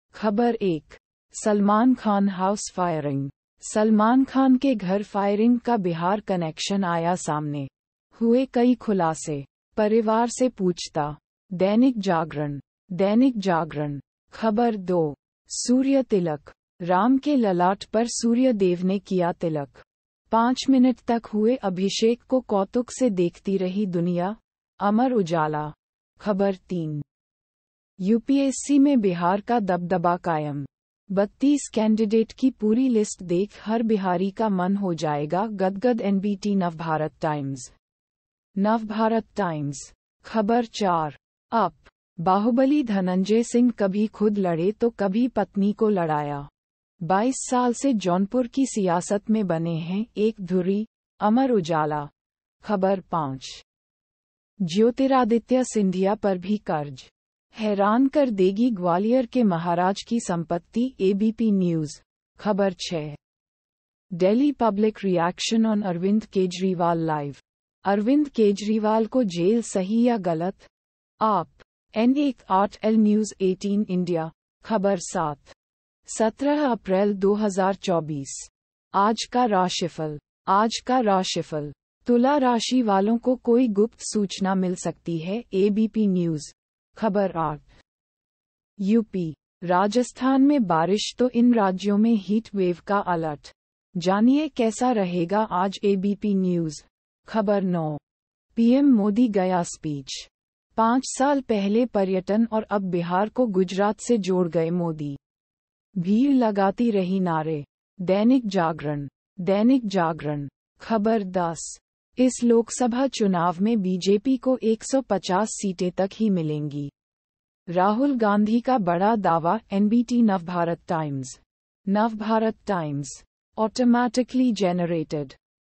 Hindi News (भारत)- 17/04/2024,01:09 PM GMT+5:30
ख़बर 1:Salman Khan House Firing: सलमान खान के घर फायरिंग का बिहार कनेक्शन आया सामने, हुए कई खुलासे; परिवार से पूछता.. - दैनिक जागरण (Dainik Jagran)
ख़बर 2:सूर्य तिलक: राम के ललाट पर सूर्य देव ने किया तिलक, पांच मिनट तक हुए अभिषेक को कौतुक से देखती रही दुनिया - अमर उजाला
ख़बर 3:UPSC में बिहार का दबदबा कायम, 32 कैंडिडेट की पूरी लिस्ट देख हर बिहारी का मन हो जाएगा गदगद - NBT नवभारत टाइम्स (Navbharat Times)
ख़बर 4:UP: बाहुबली धनंजय सिंह कभी खुद लड़े तो कभी पत्नी को लड़ाया... 22 साल से जौनपुर की सियासत में बने हैं एक धुरी - अमर उजाला
ख़बर 5:ज्योतिरादित्य सिंधिया पर भी कर्ज, हैरान कर देगी ग्वालियर के महाराज की संपत्ति - ABP न्यूज़
ख़बर 6:Delhi Public Reaction on Arvind Kejriwal Live : अरविंद केजरीवाल को जेल सही या गलत ? | AAP । N18L - News18 India
ख़बर 7:17 अप्रैल 2024, आज का राशिफल (Aaj ka Rashifal): तुला राशि वालों को कोई गुप्त सूचना मिल सकती है - ABP न्यूज़
ख़बर 8:यूपी, राजस्थान में बारिश तो इन राज्यों में हीट वेव का अलर्ट, जानिए कैसा रहेगा आज - ABP न्यूज़
ख़बर 9:PM Modi Gaya Speech: 5 साल पहले पर्यटन और अब बिहार को गुजरात से जोड़ गए मोदी, भीड़ लगाती रही नारे.. - दैनिक जागरण (Dainik Jagran)
ख़बर 10:इस लोकसभा चुनाव में बीजेपी को 150 सीटें तक ही मिलेंगी, राहुल गांधी का बड़ा दावा - NBT नवभारत टाइम्स (Navbharat Times)

Automatically Generated...